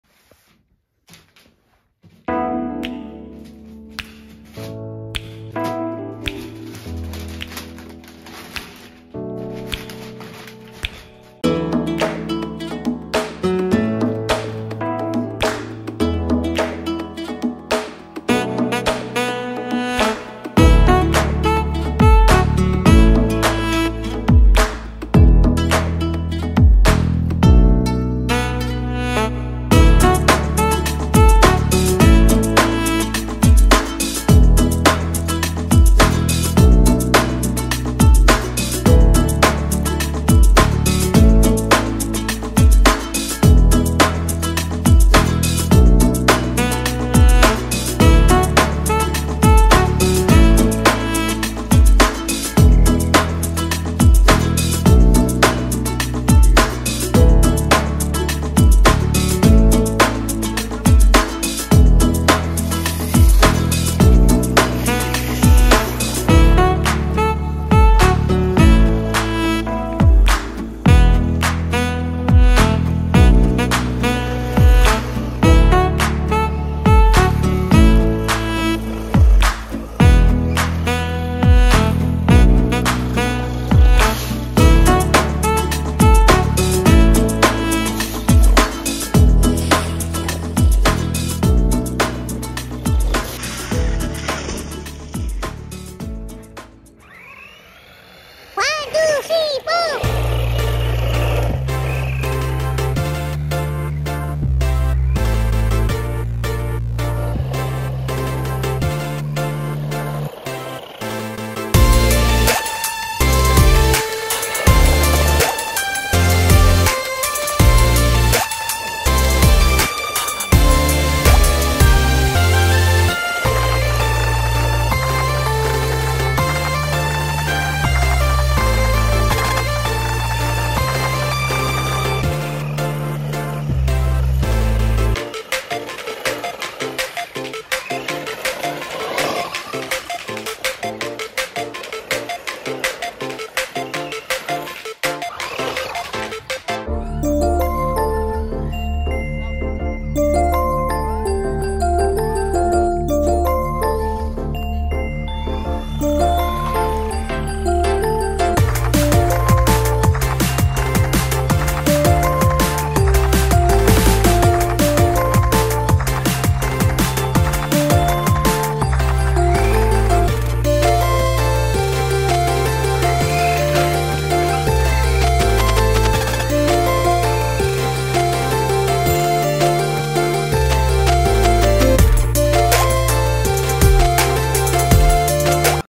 Déballage du cadeau et premier essai du Monster truck de Funtek. (Mtx)
Essai à 25 et 50km.